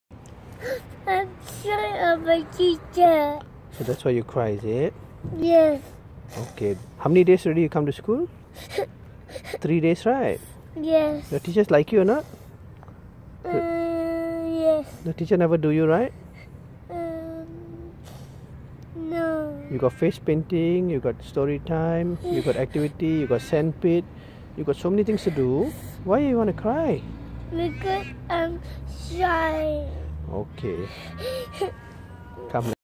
Because I am shy
My son Gavin when he was just over 3 years old. We arrived in Perth and I was doing my best to gently coax him from the car, to go to daycare.